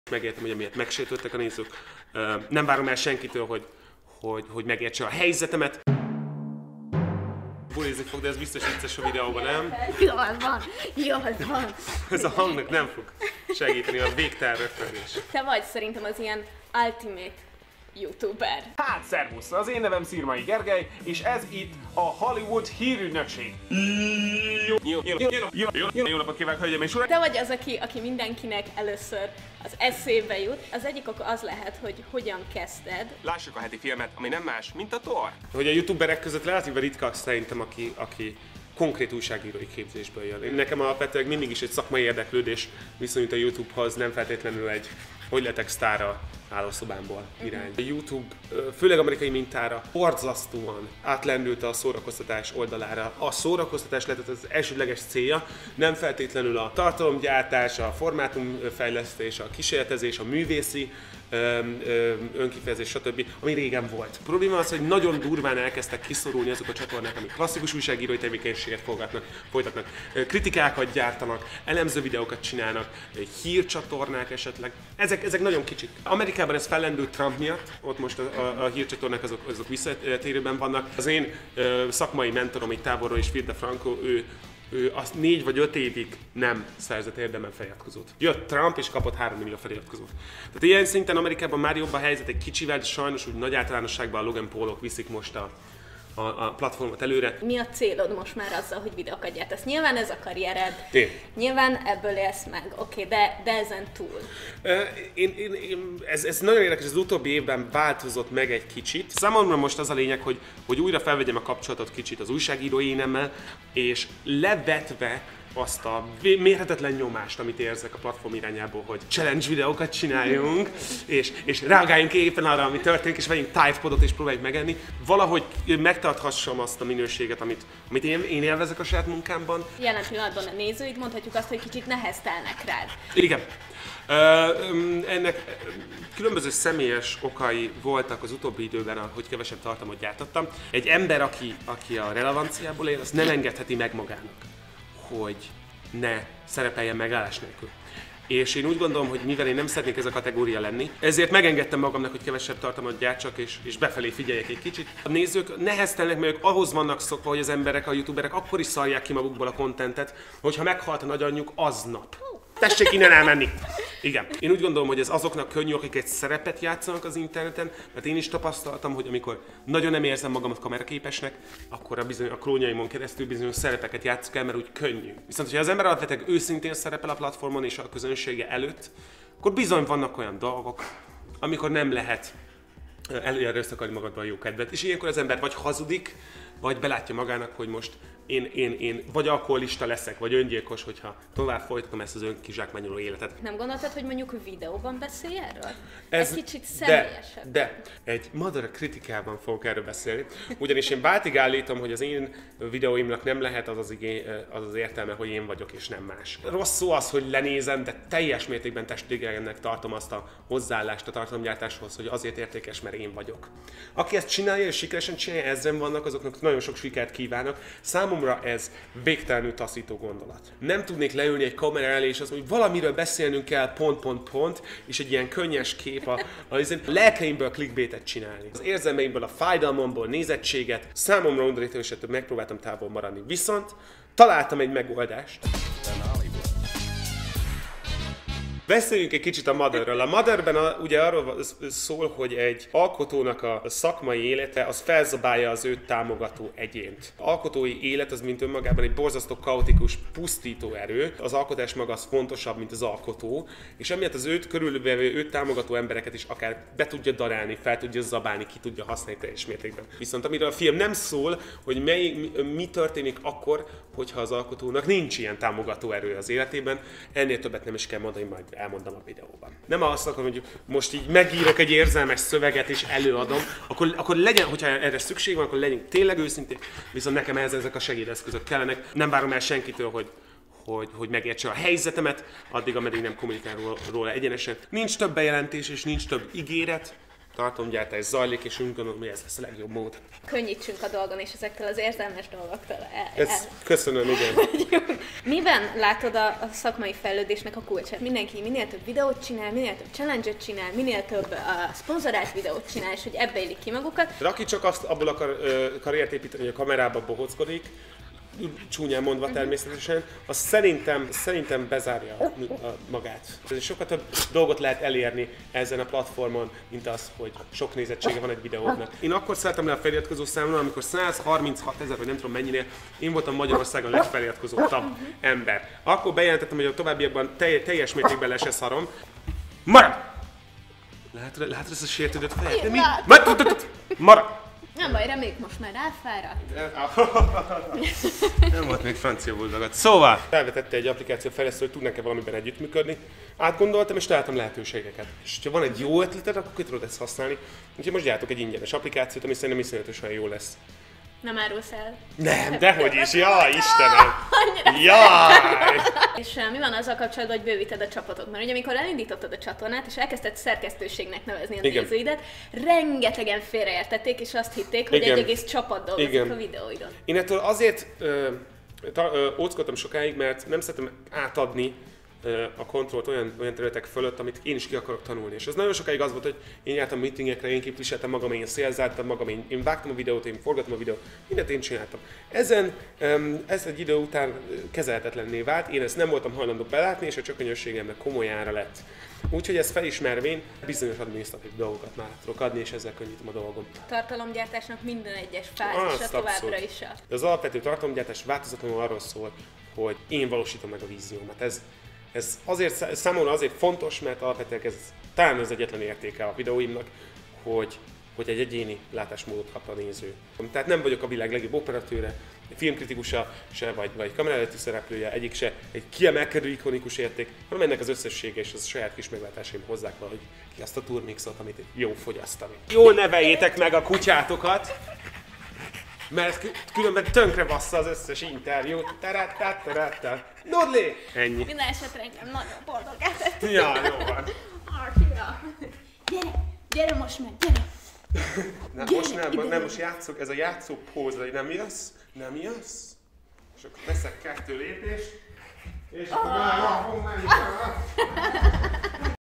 Szirmai Gergő: Nem akarok a lelkemből clickbaitet csinálni | Zoom.hu
Interjúztunk Magyarország talán legismertebb youtuberével, a Hollywood Hírügynökséges Szirmai Gergővel. Beszélgettünk vele eltűnéséről, a nézőkkel való viszonyáról és a videókészítés mögött álló motivációkról.

Készítette: László Viktória

További tartalmainkat kövesd itt!